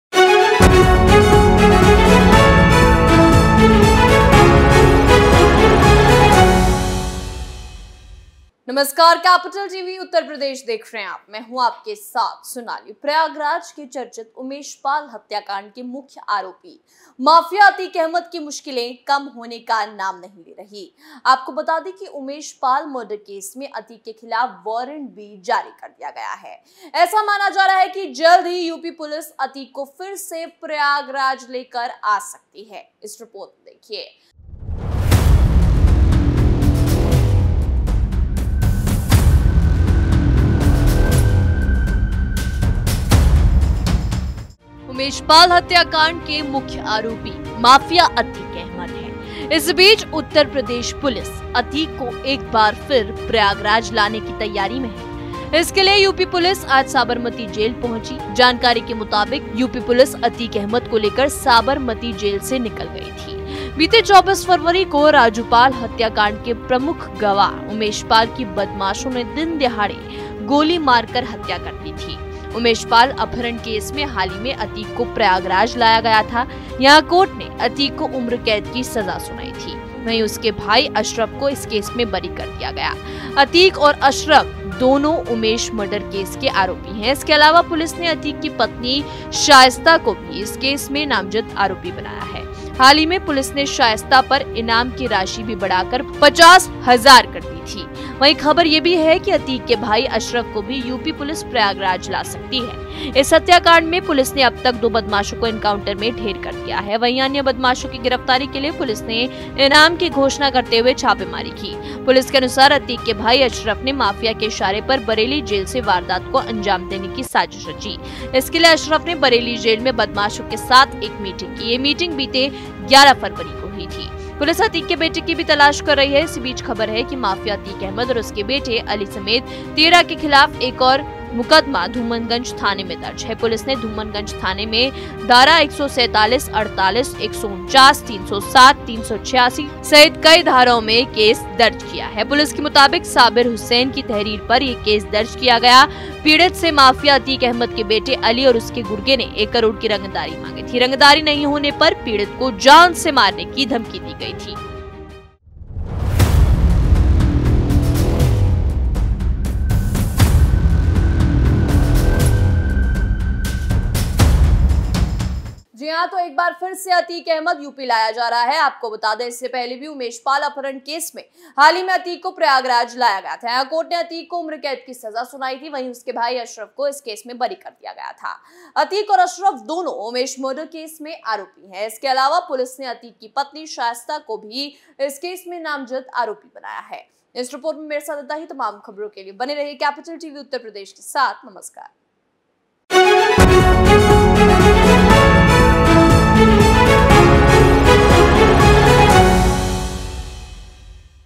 Jail में बंद अतीक अहमद को फिर ला रही UP पुलिस | Capital TV Uttar Pradesh
Mafia don Atiq Ahmed was again sent to Gujarat's Sabarmati Jail after being sentenced to life imprisonment in the Umesh Pal kidnapping case of Prayagraj. But now there is news that he is again being brought to Prayagraj. The UP Police reached Sabarmati Jail this morning to pick up Atiq Ahmed.